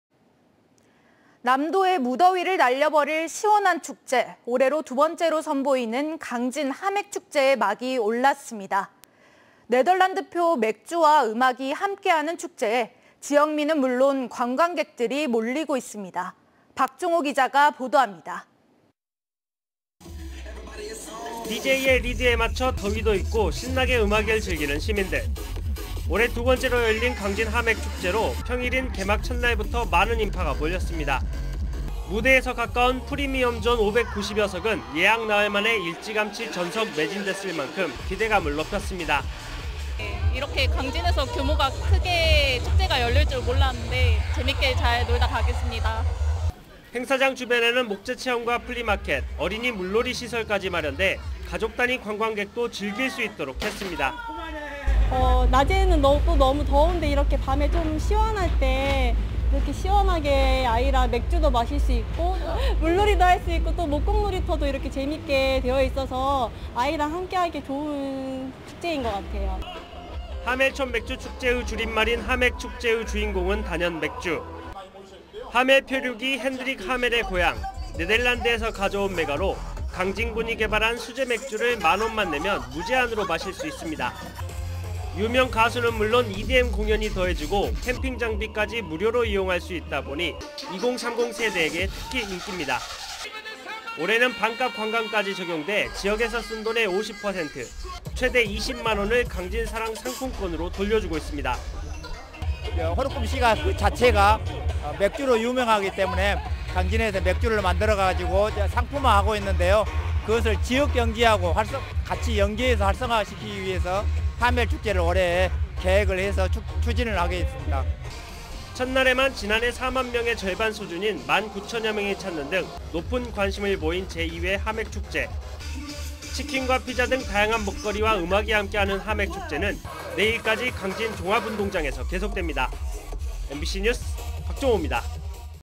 네덜란드의 맛을 강진에서..하맥축제 '들썩' -R(240830금/뉴스데스크)
남도의 무더위를 날려버릴 시원한 축제, 올해로 두번째로 선보이는
강진 '하맥축제'의 막이 올랐습니다.
네덜란드표 맥주와 음악이 함께하는 축제에
지역민은 물론 관광객들이 몰리고 있습니다.
박종호 기자가 보도합니다.

◀ 리포트 ▶

DJ의 리드에 맞춰 더위도 잊고
신나게 음악을 즐기는 시민들.

올해 두 번째로 열린 강진 하맥축제로
평일인 개막 첫 날부터
많은 인파가 몰렸습니다.

무대에서 가까운 '프리미엄존' 590여석은
예약 나흘 만에 일찌감치 전석 매진됐을 만큼
기대감을 높였습니다.

◀ INT ▶ 백지선 광주광역시
"강진에서 규모가 크게 축제가 열릴 줄 몰랐는데 재밌게 잘 놀다 가겠습니다."

행사장 주변에는 목재체험과 플리마켓,
어린이 물놀이 시설까지 마련돼
가족 단위 관광객들도 즐길 수 있도록
했습니다.

◀ INT ▶강효선 강진군
낮에는 너무 더운데 이렇게 밤에 시원할 때 이렇게 시원하게 아이랑 맥주도 마실 수 있고 또 아이는 물놀이도 할 수 있고 또 목재 놀이터도 이렇게 재미있게 되어 있어서 아이랑 함께하기에 좋은 축제인 것 같아요.

하멜촌맥주축제의 줄임말인
'하맥축제'의 주인공은 단연 맥주.

하멜 표류기의 '핸드릭 하멜'의 고향,
네덜란드에서 가져온 맥아로
강진군이 개발한 수제 맥주를
1만 원만 내면 무제한으로 마실 수 있습니다.

유명 가수는 물론 EDM 공연이 더해지고,
캠핑 장비까지 무료로 이용할 수 있다보니
2030세대에게 특히 인기입니다.

올해는 '반값관광'까지 적용돼
지역에서 쓴 돈의 50%, 최대 20만 원을
강진사랑상품권으로 돌려주고 있습니다.

◀ INT ▶김진관 강진군 축제마케팅추진단장
(네덜란드) 호르큼시는 맥주로 유명하기 때문에 강진에서는 맥주를 만들어서 상품화하고 있는데요. 저희들 지역 경기하고 같이 연계해서 활성화시키기 위해서 하맥 축제를 올해 추진을 했습니다.

첫날에만 지난해 4만여 명의
절반 수준인 1만9천여 명이 찾는 등
높은 관심을 보인 제2회 하맥축제.

치킨과 피자 등 다양한 먹거리와
음악이 함께하는 하맥축제는 내일(31일)까지
강진종합운동장에서 계속됩니다.

MBC뉴스 박종호입니다.